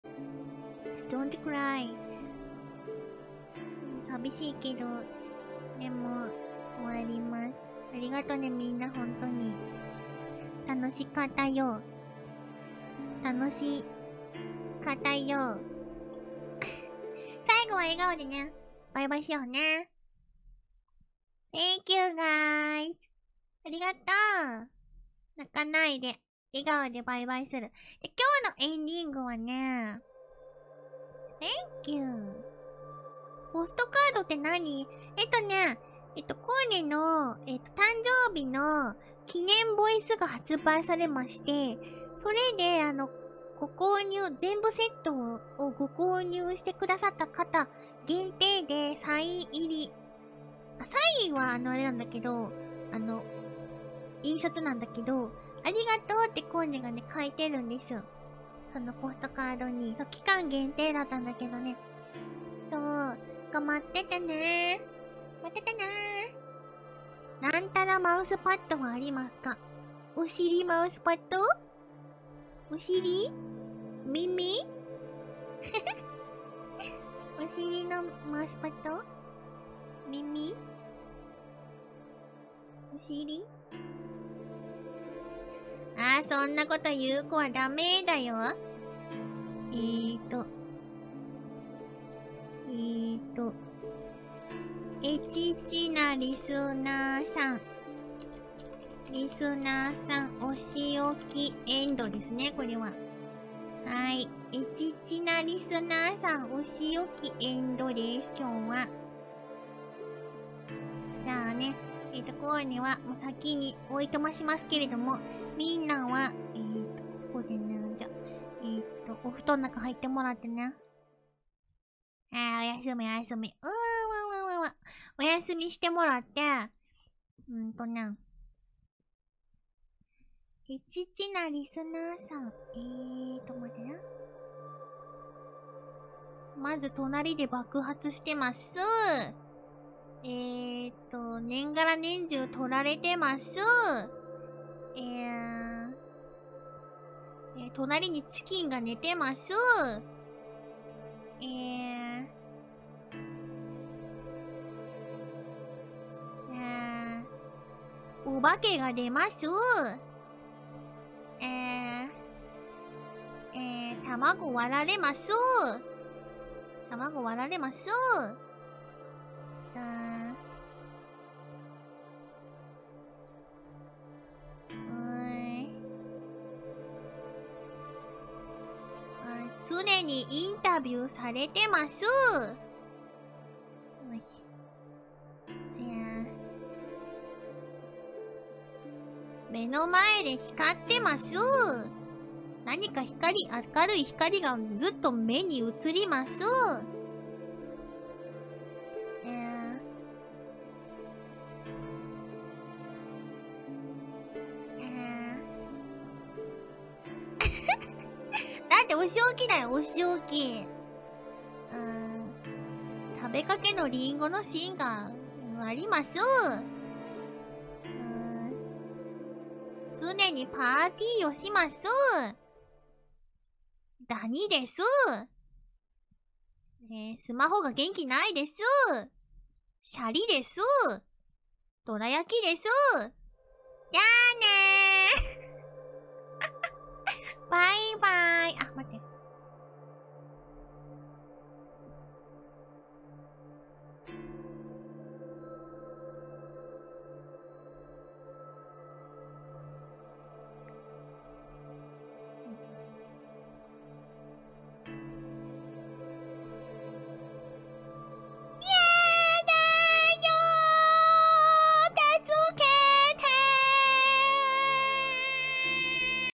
Korone Punishes Chat for Being Too H0rny [Eng Subs/Hololive]
Hilariously, I looked through the chat just to make sure, and I don't think anyone even mentioned "Booty Mousepad" until Korone did. She's punishing chat for a crime they didn't even commit! Poor Listener-san...